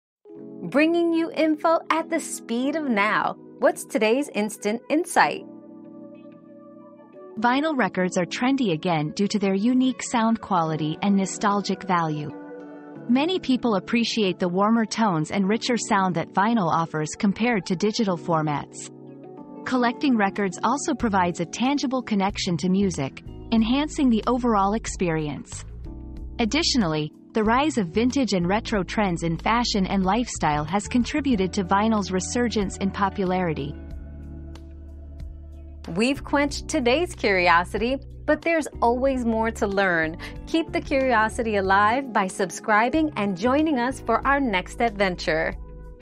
Why are vinyl records trendy again?
The Resurgence of Vinyl Records: Why They're Trendy Again 👉 Vinyl Revival 👉 Discover why vinyl records are making a comeback, from their unique sound quality to their nostalgic appeal. Dive into the world of collecting records and the influence of vintage trends on their resurgence in popularity.

Laura S. Harris (2024, June 22.) Why are vinyl records trendy again?
    🌐 AskAbout.video